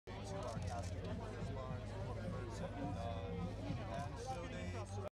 Kate Flannery at Audi And Derek Lam Kick Off Emmy Week 20...
Kate Flannery at Audi And Derek Lam Kick Off Emmy Week 2012 on 9/16/12 in Los Angeles, CA